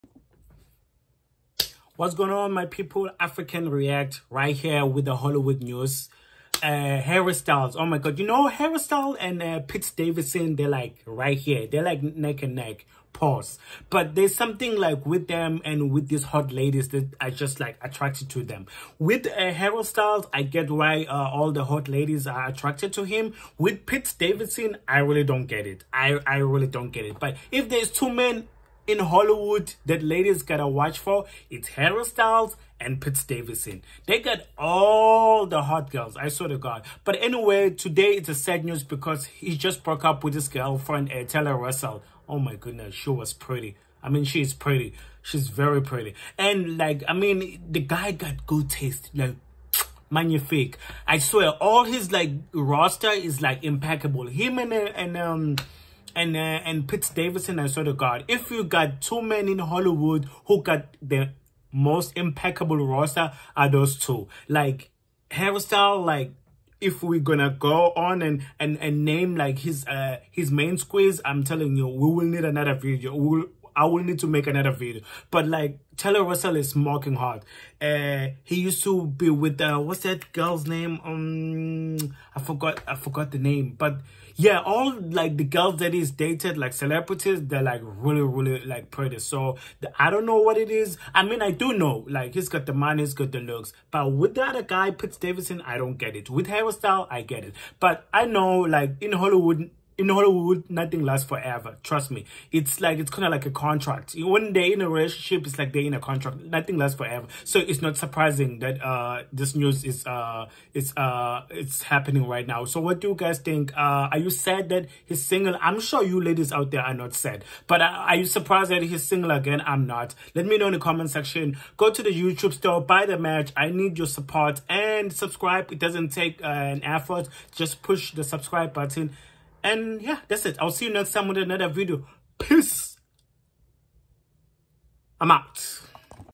HARRY STYLES IS SINGLE AGAIN🤦‍♂️(NOW PRETEND TO BE SURPRISE) THE CONTRACT EXPIRED🤣🤣🤣
Harry Styles is single again. I’m not surprised at all. The contract expired already, you know the Hollywood way🤣🤣🤣.